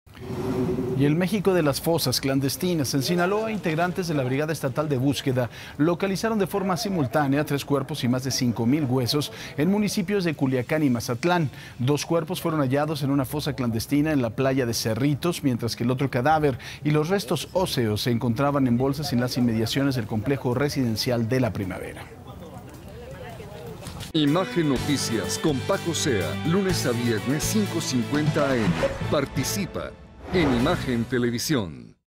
Localizan 3 cuerpos y más de 5 mil huesos en Sinaloa | Noticias con Francisco Zea
¡El México de las fosas clandestinas! Integrantes de la Brigada Estatal de Búsqueda localizaron tres cuerpos y más de cinco mil huesos en Sinaloa.